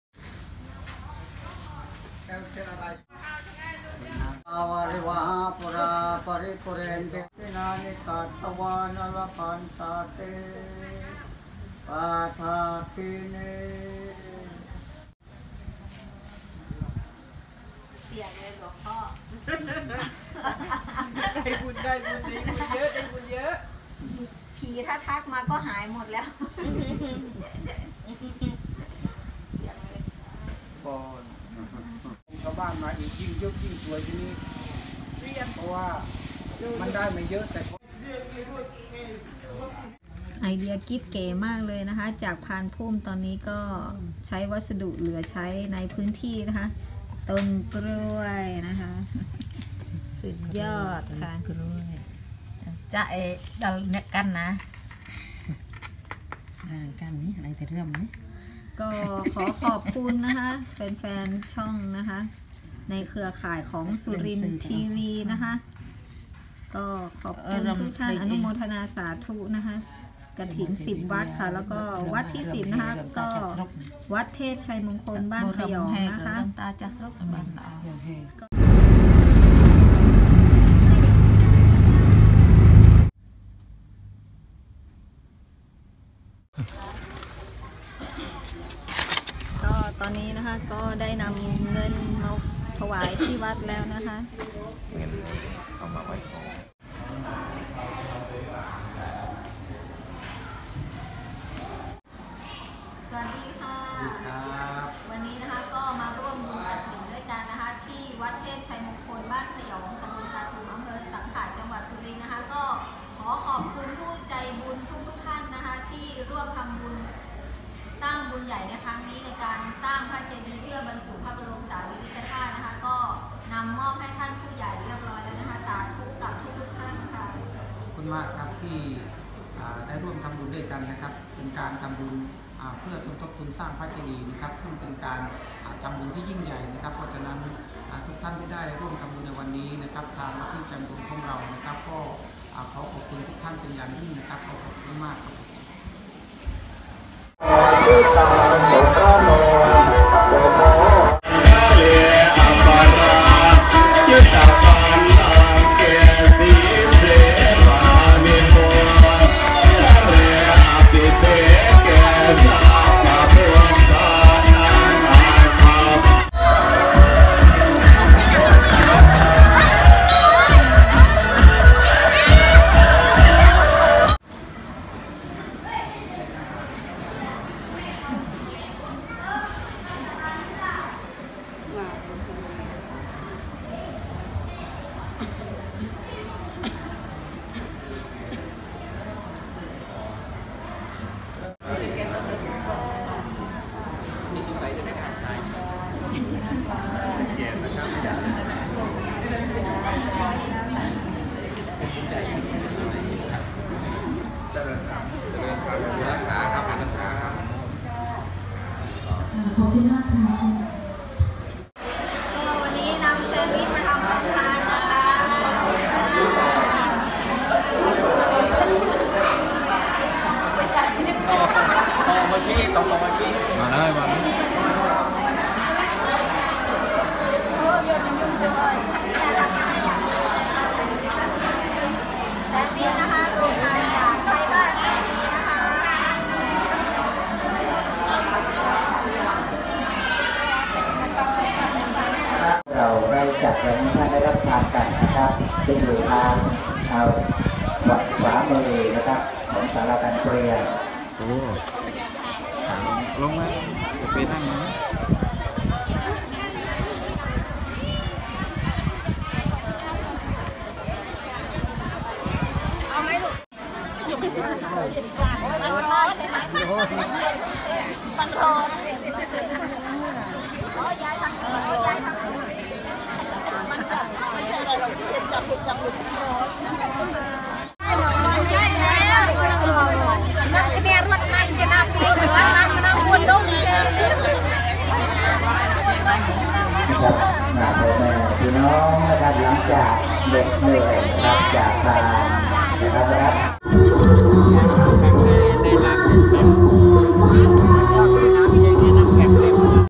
สร้างบุญกฐินวัดเทพไชยมงคลเพื่อสร้างพระบรมสารีริกธาตุ ส่งบุญให้ผีเรือล่มทั้ง5
กฐินวัดเทพไชยมงคลบ้านขยอง ยอด 120,560บาท อนุโมทนาสาธุกับทุกๆท่าน